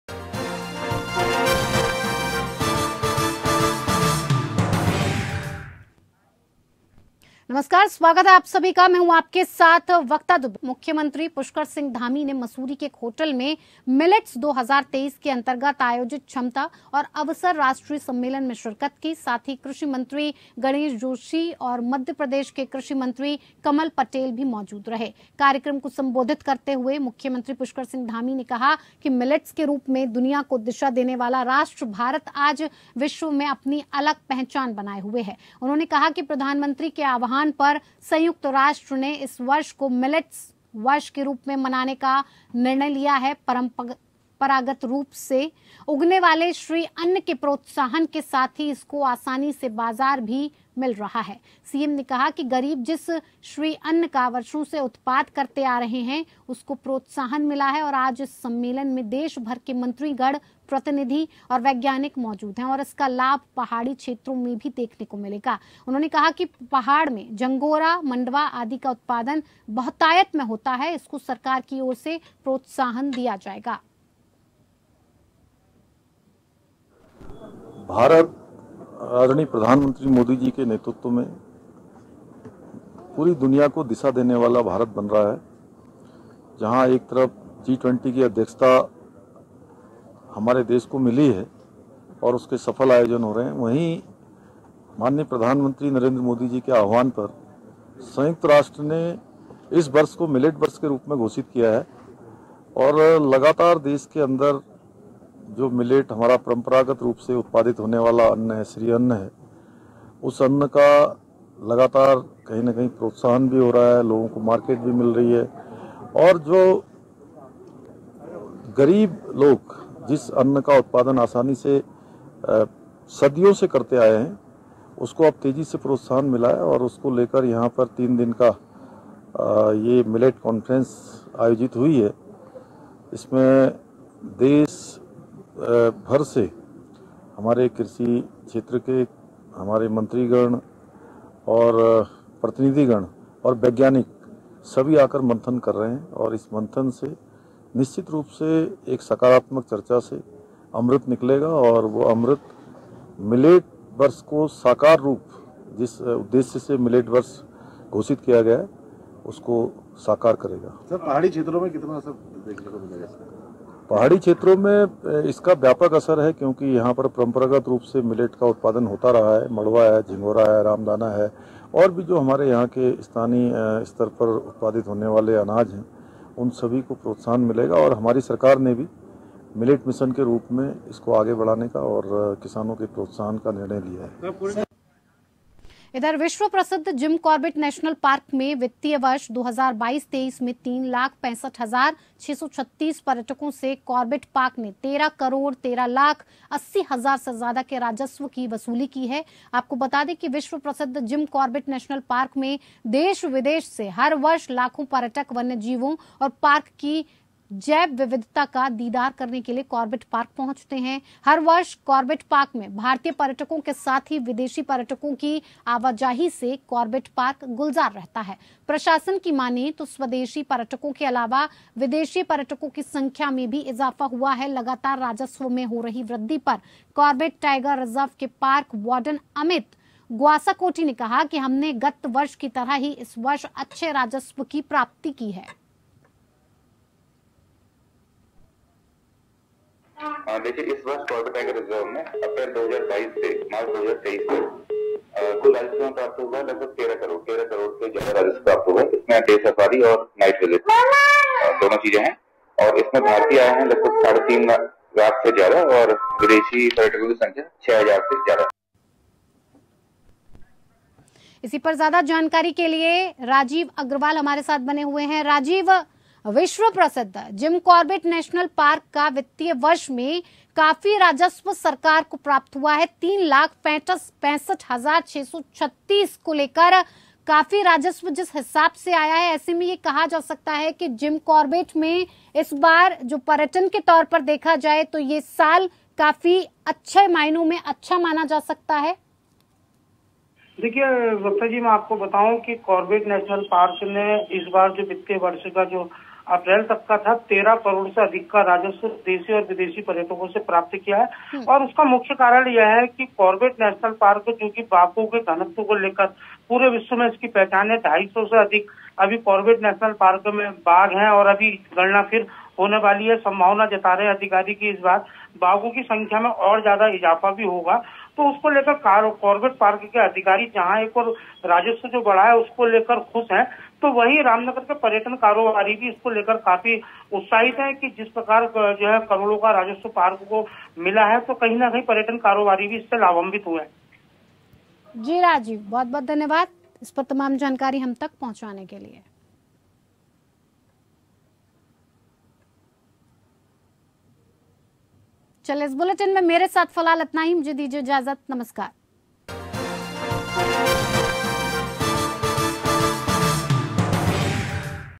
Uttarakhand latest news :मिलेट्स 2023 सम्मेलन का आयोजन। Organization of Millets 2023 conference.
मिलेट्स 2023 के अंतर्गत क्षमता और अवसर सम्मेलन का आयोजन किया गया। सीएम धामी ने की कार्यक्रम में शिरकत । सीएम धामी ने कहा- मिलेट्स को दिया जाएगा प्रोत्साहन । प्रशासन के मुताबिक विदेशी पर्यटकों की भी संख्या बढ़ी है।

Capability and Opportunity Conference was organized under Millets 2023. CM Dhami participated in the program. CM Dhami said – Millets will be encouraged. According to the administration, the number of foreign tourists has also increased.